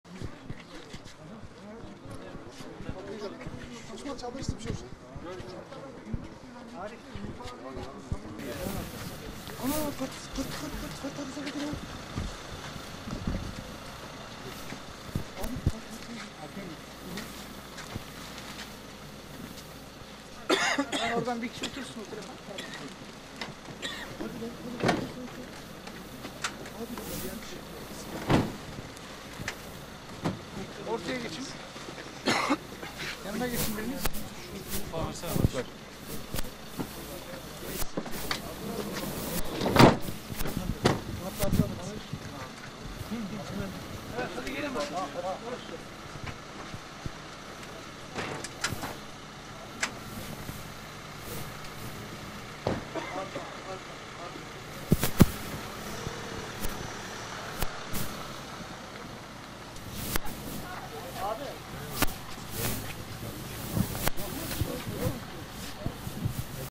Usulsüz dinleme operasyonu: 4 tutuklu cezaevine gönderildi
Ankara Gölbaşı Cumhuriyet Başsavcılığı’nın TÜBİTAK ve TİB'e yaptığı usulsüz dinleme operasyonu sonrası mahkeme tarafından tutuklanan 4 kişi cezaevine gönderildi.